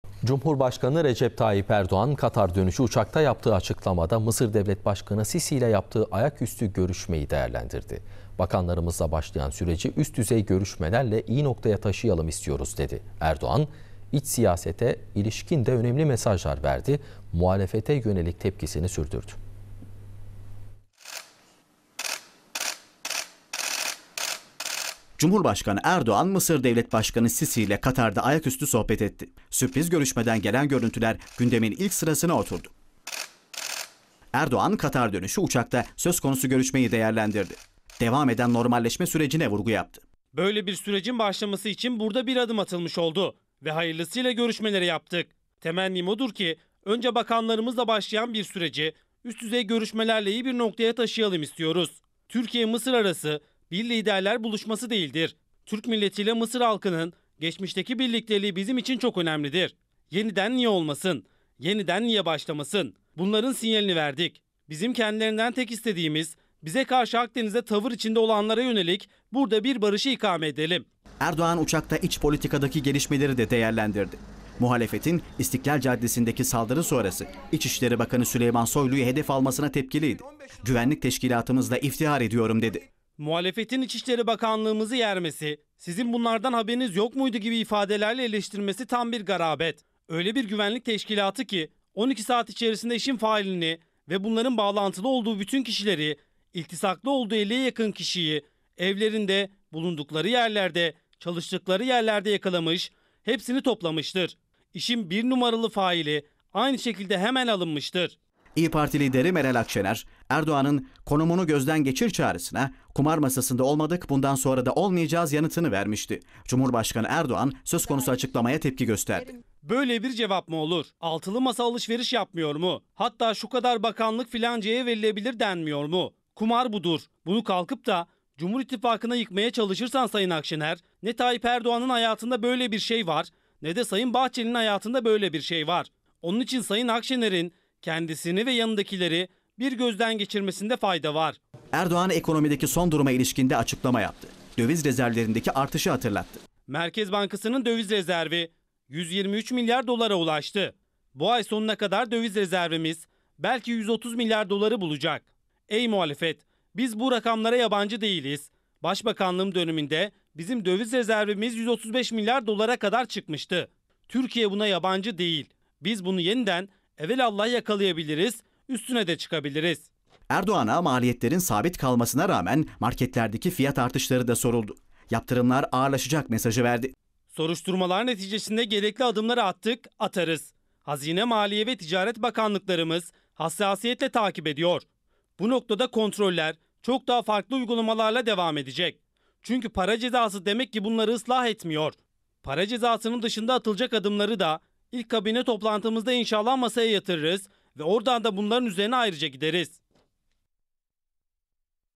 Erdoğan Fırsatçıları Uyardı: Madem Para Cezası Islah Etmiyor...
Katar dönüşü uçakta basın mensuplarının sorularını yanıtlayan Cumhurbaşkanı Erdoğan, maliyet fiyatlarının sabit kalmasına rağmen sürekli fiyatlarda artışa giden fırsatçıları uyardı. Erdoğan, "Madem para cezaları bunları ıslah etmiyor. Bunun dışında atılacak adımları ilk Kabine toplantısında masaya getireceğiz" dedi.